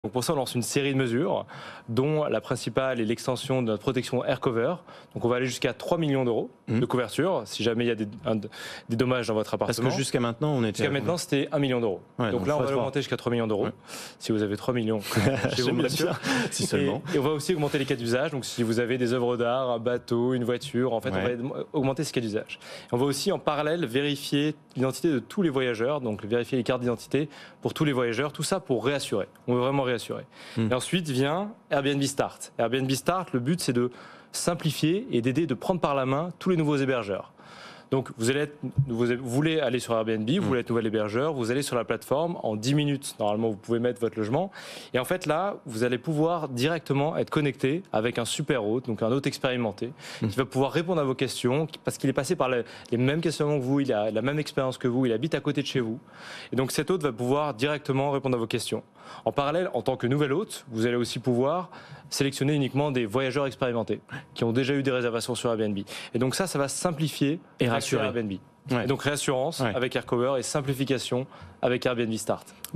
.@airbnb_fr annonce de nouvelles mesures et Airbnb Start
"Réassurance avec AirCover (de 1 à 3M€) et simplification avec Airbnb Start" Bertrand Brudet